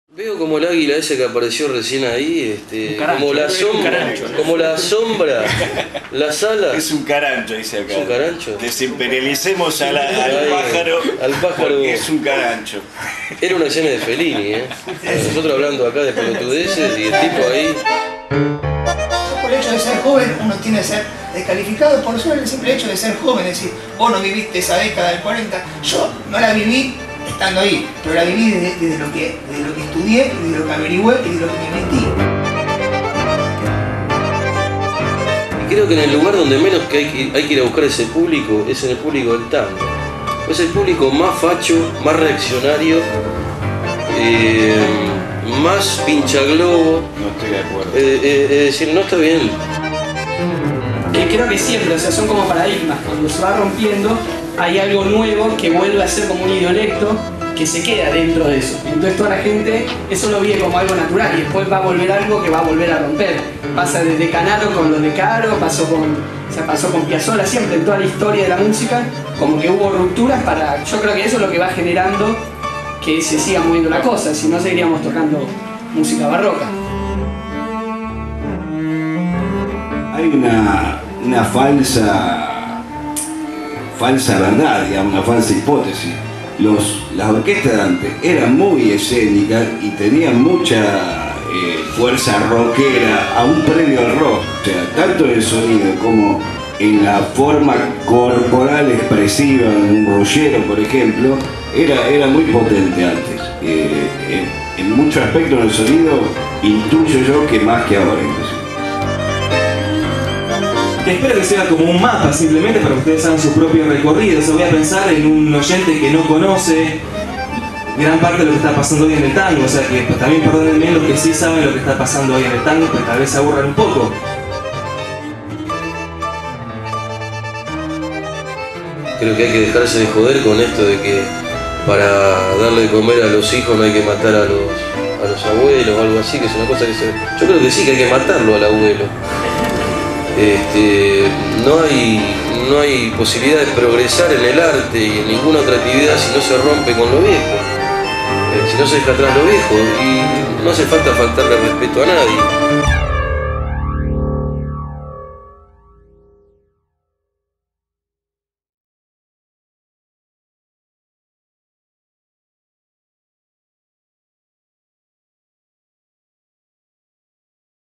TRAILER - Tango en el siglo XXI Mesa Debate
19/08/2010 Avance sobre la cobertura de una de las mesas de discusión más jugosas sobre el tango, vaya uno a saber desde cuando. Cuatro músicos, un poeta y un periodista aportando a la discusión del tango en la actualidad. Coordinado por el portal en el marco del XII Festival de Tango de la Ciudad de Buenos Aires.